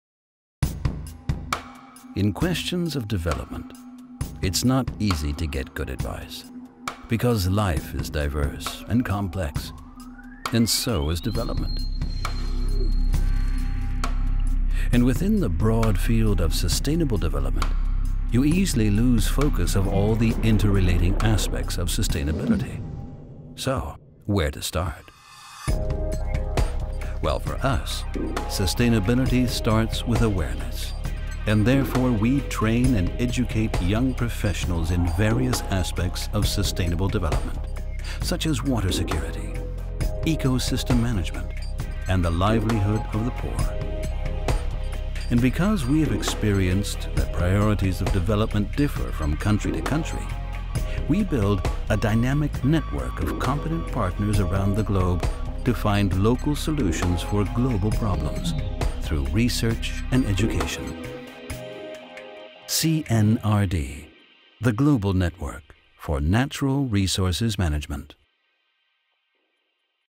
CNRD - Centers for Natural Resources and Development
CNRD is a worldwide university network promoting academic exchange and cooperation in the area of natural resources management, in particular related to water, land, ecosystem and renewable energy resources. The network aims to provide a valuable contribution to the Post 2015 Agenda and to the Sustainable Development Goals (SDG). More information on cnrd.info